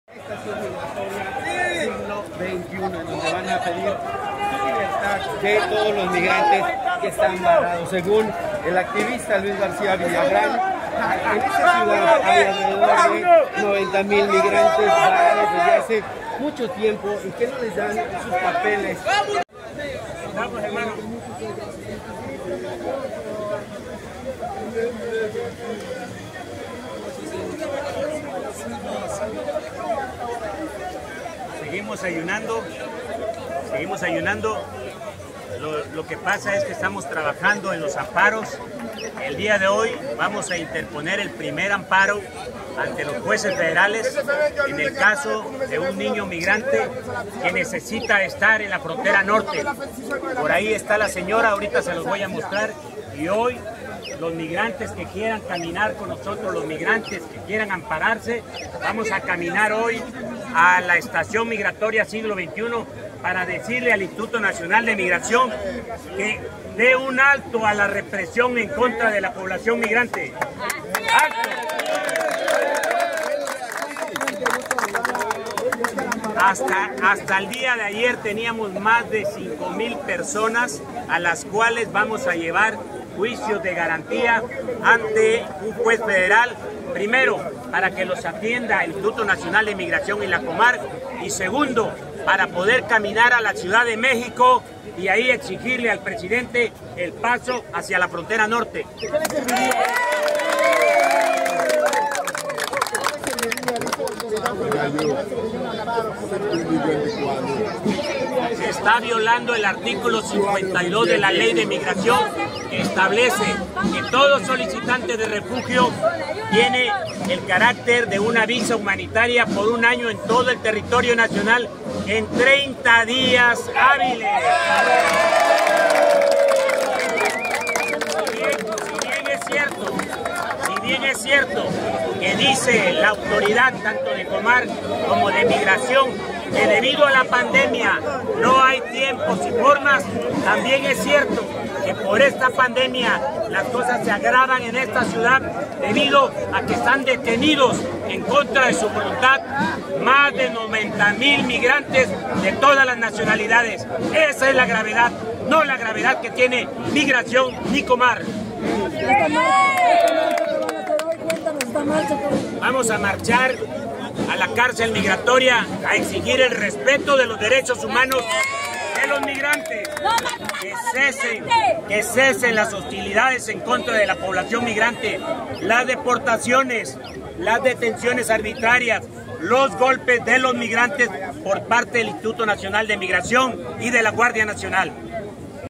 Migrantes marchan “Por la libertad” en Tapachula
Unos 300 migrantes mayormente haitianos realizaron una marcha protesta por la falta de atención a sus trámites migratorios.  También denunciaron hostigamiento y represión del Ayuntamiento que encabeza Rosa Irene Urbina Castañeda.